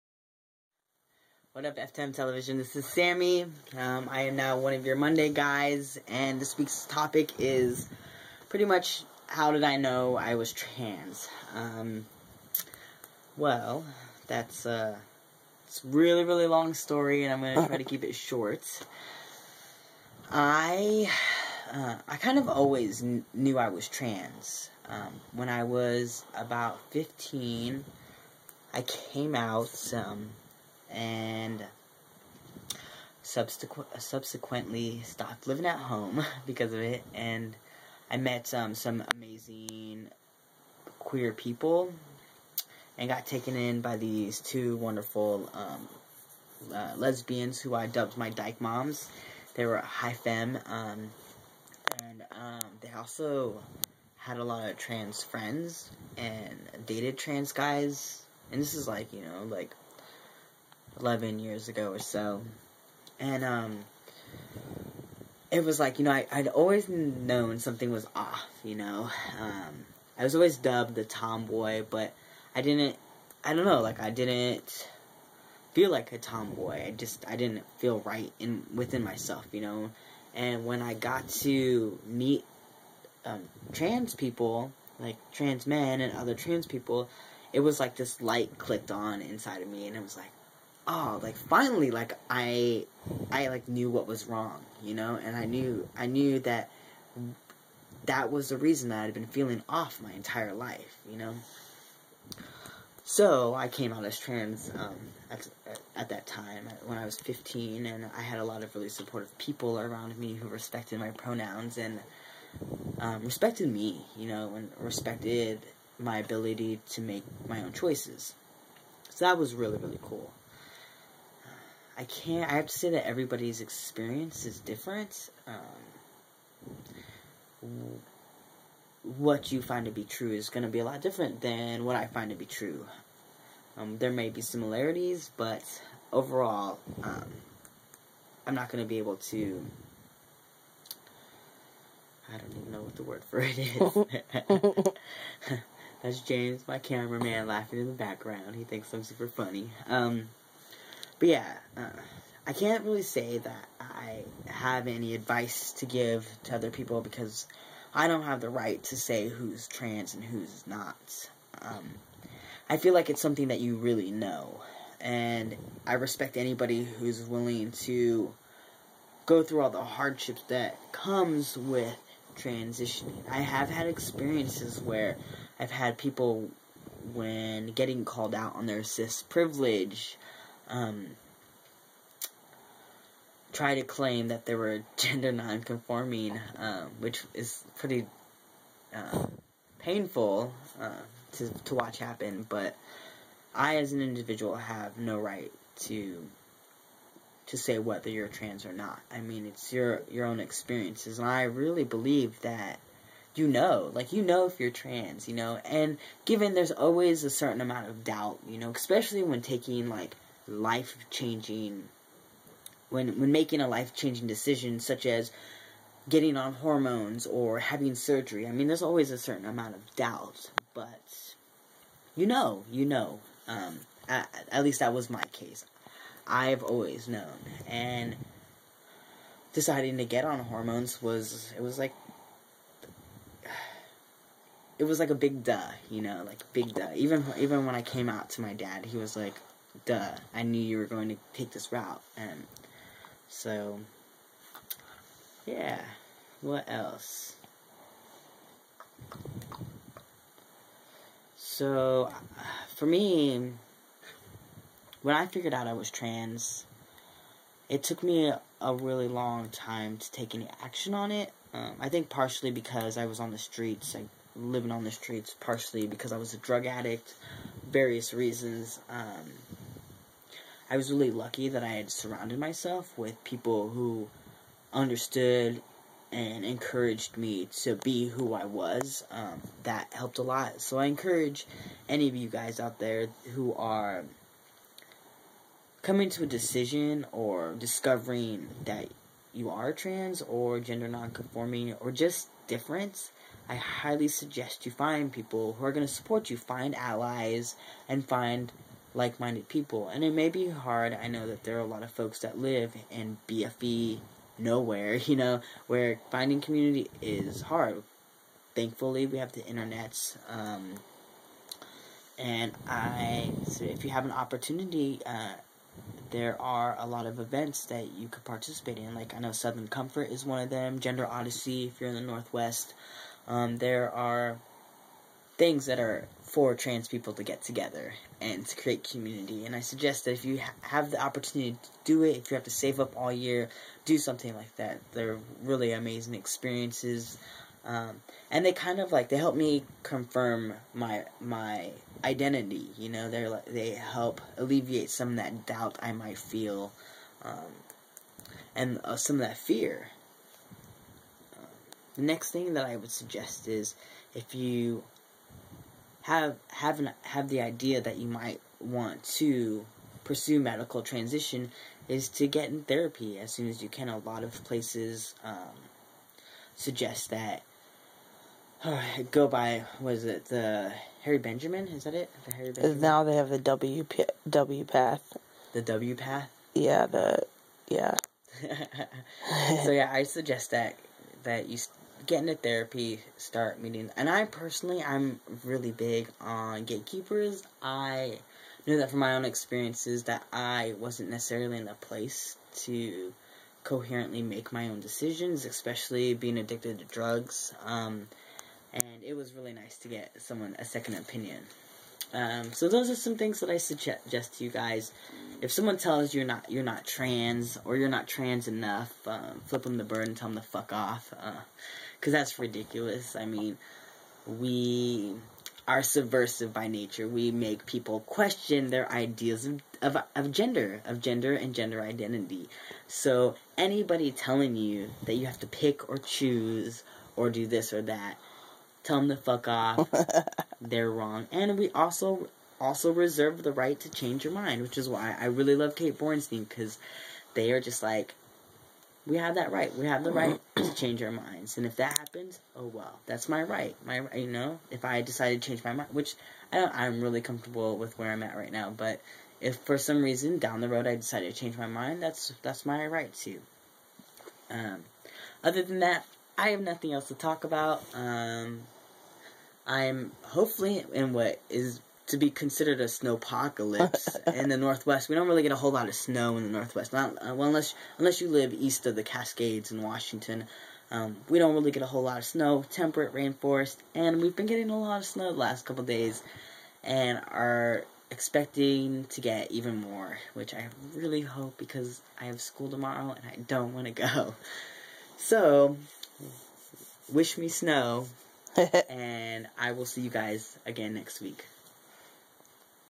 FTMtv Week 33-Sammy- OMG I'm TRANS*? How I found out.
Happy Martin Luther King Jr. Day!

My story about how I discovered I was trans* and some advice for newer guys out there.